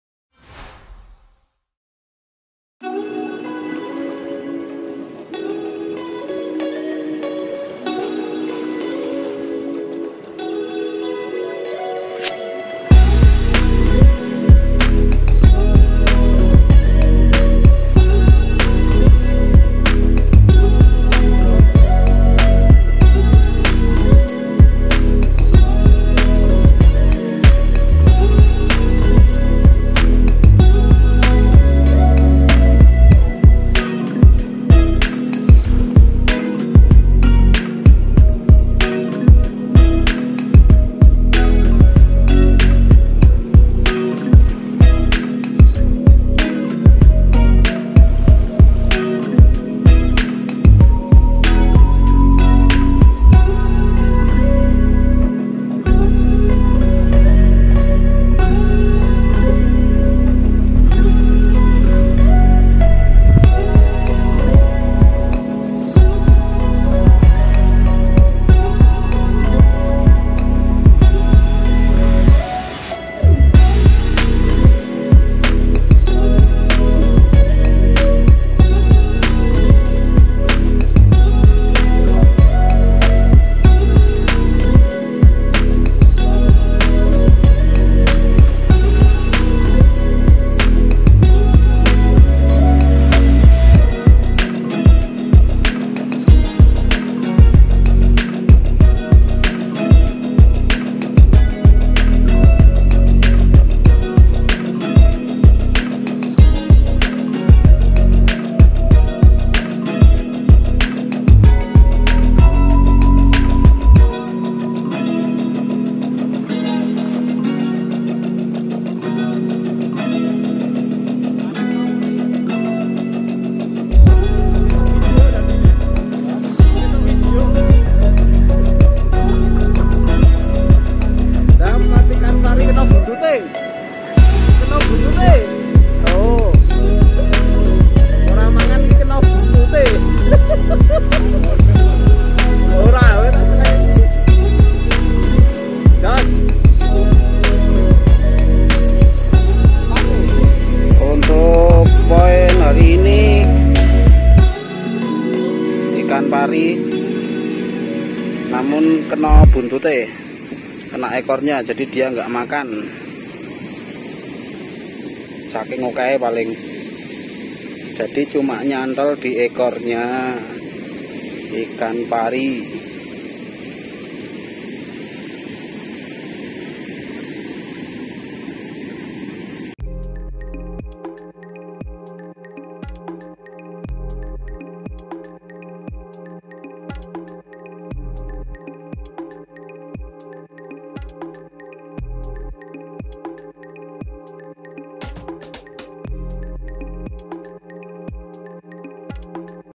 SURF FISHING !!! MANCING PASIRAN TERBARU DI PANTAI SELATAN JAWA JOGJA STRIKE IKAN PARI
kali ini saya mancing ikan di pinggir laut / surf fishing / mancing pasiran.
untuk lokasi saya di selatan basecame pasiraner bantul jogjakarta.
mancing sambil menikmati indahnya pemandangan sunset.
mancing pasiran terbaru ini saya abadikan.

joran hemus barracuda, reel tridentech

2. If you have any questions, please comment.